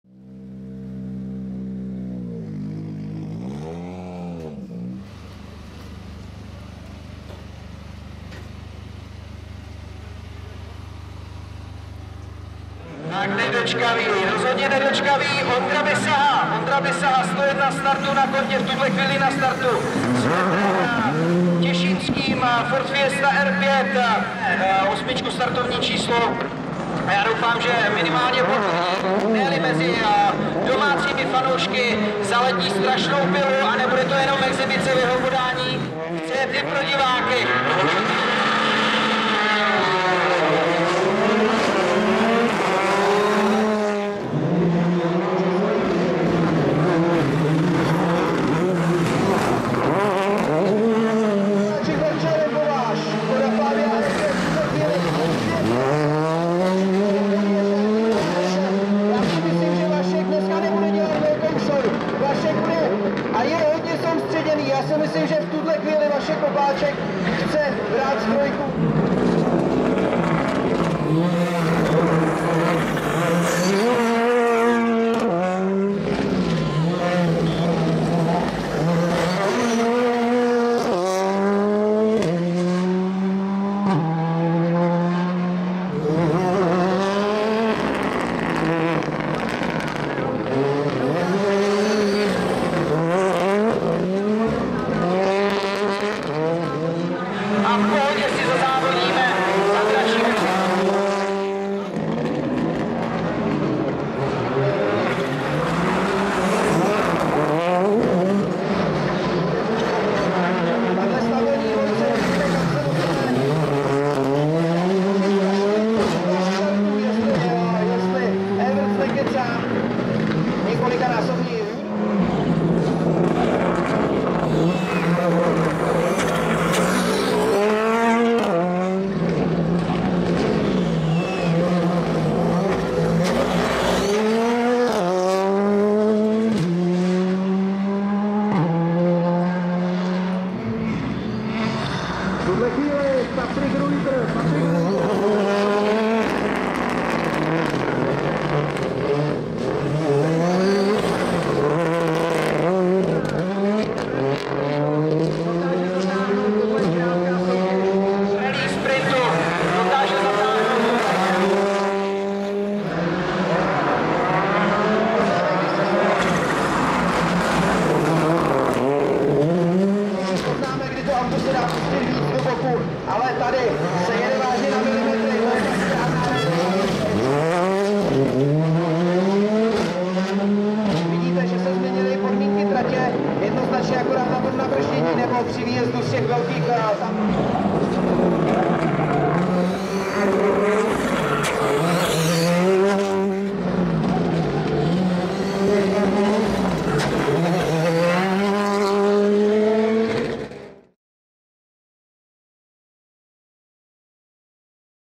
Ford Fiesta R5 - Ondřej Bisaha / Petr Těšínský - Mikuláš Rally IS LIFE 2018 Slušovice
Ondřej Bisaha a Petr Těšínský na voze Ford Fiesta R5 obsadili 6.místo na Mikuláš Rally 2018 Slušovice